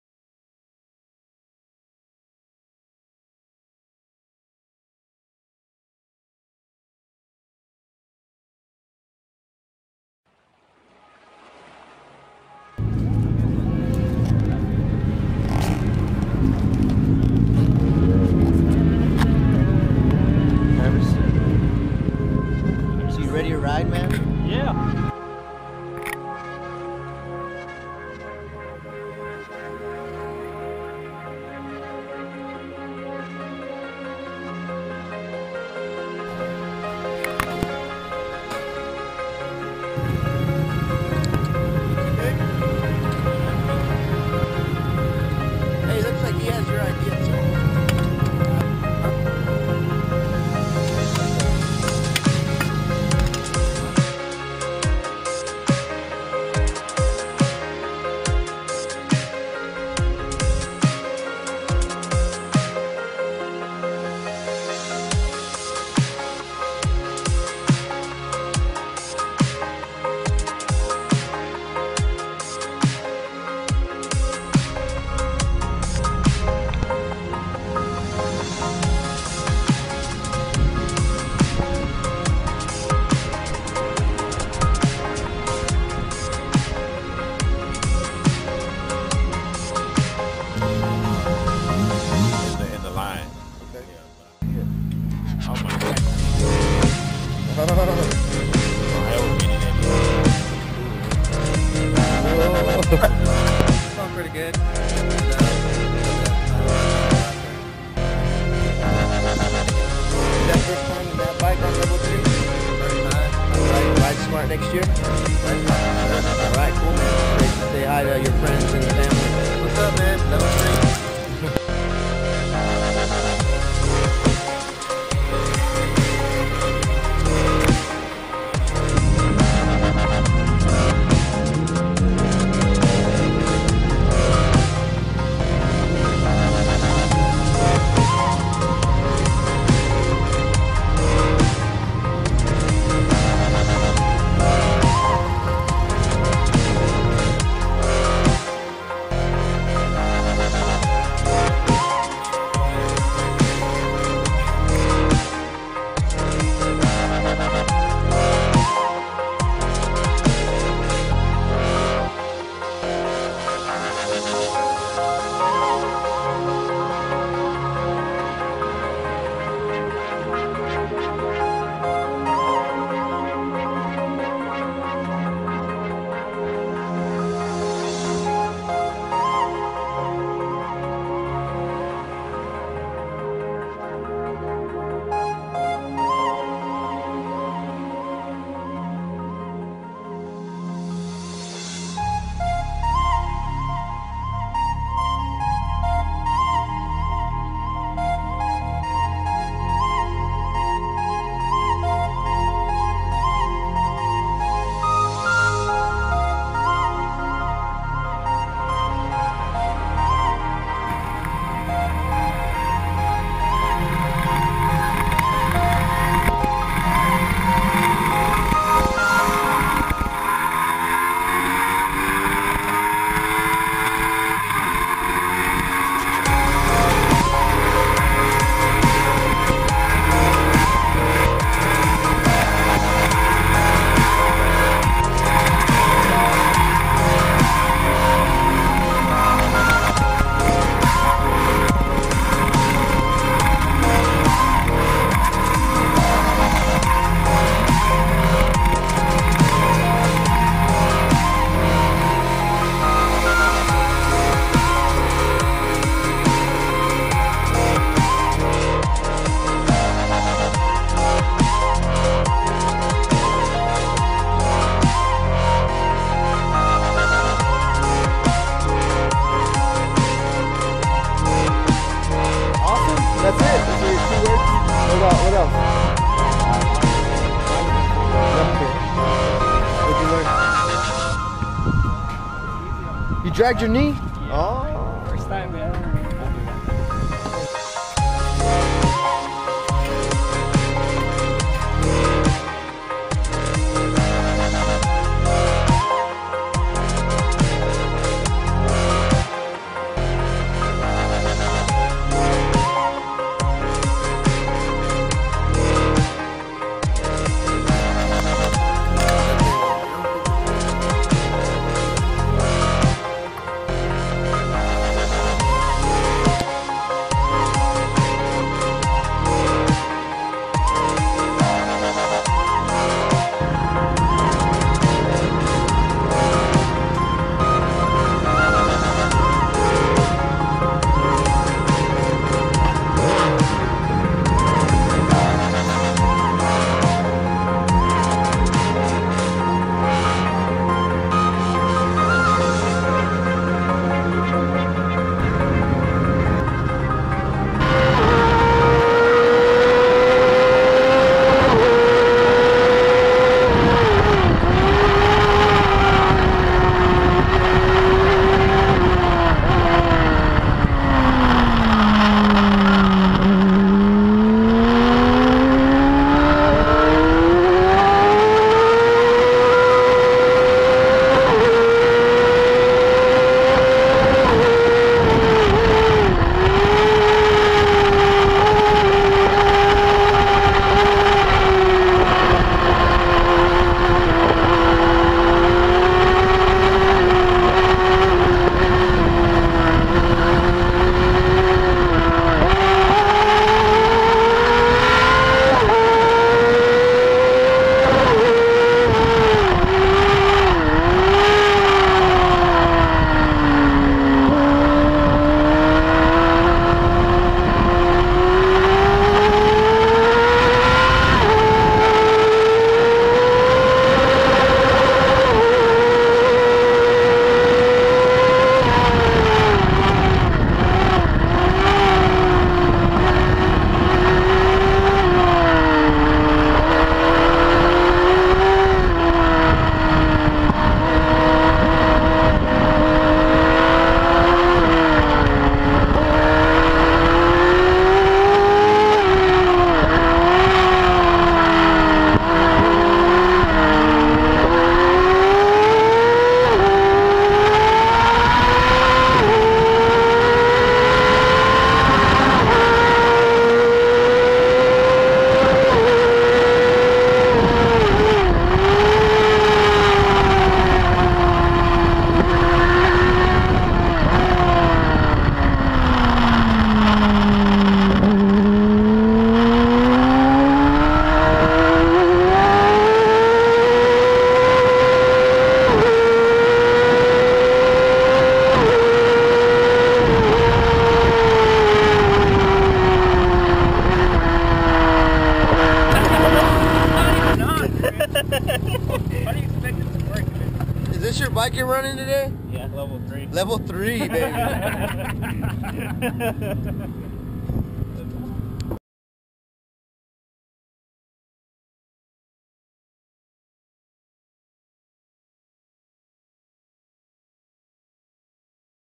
Customer Appreciation Video: MSRH Nov/9-10/13 by Ryan Short
This video includes most of the riders and friends that attended our MSR Houston event on Nov. 9-10 at Houston, TX. Thanks Blair Hart, and others for allowing us to use their photos in this video.

Do you have pictures that you have taken during the event? If so, please post them on our site. We would like to see you, your friends, your bikes and of course your family.

Thanks to all of you that make Ride Smart possible!